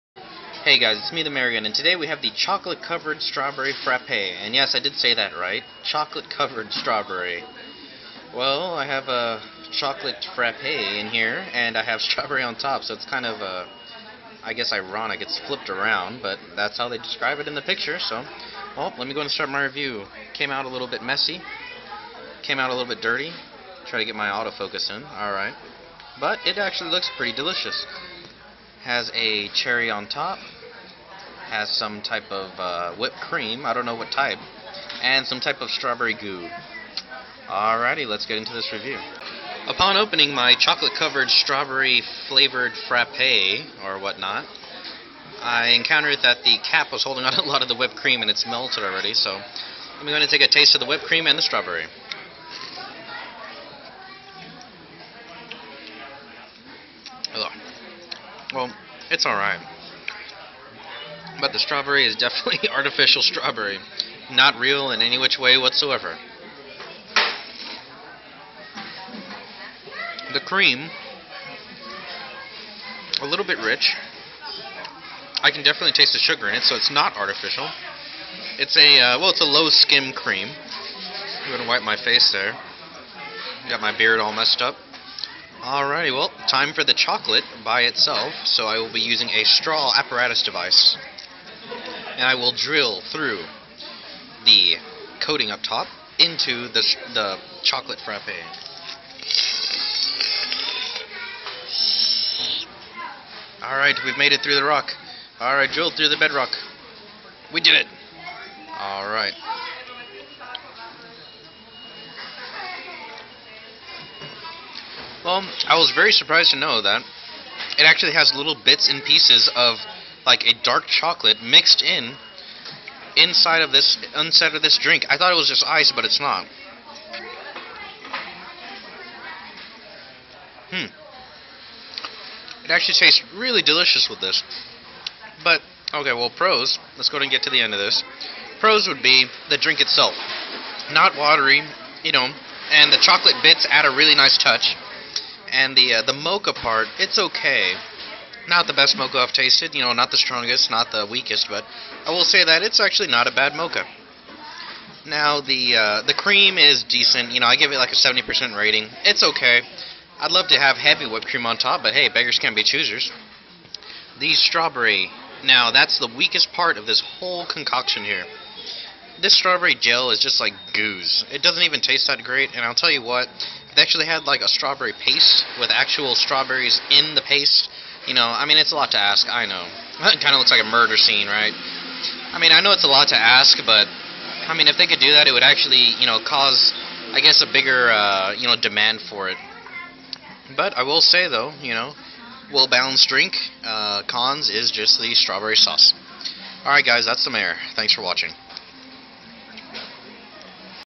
McDonalds Chocolate Covered Strawberry Frappe Mayor's Review
The Mayor tries a shake that has him clenching his stomach in joy !

Mayor Fuglycool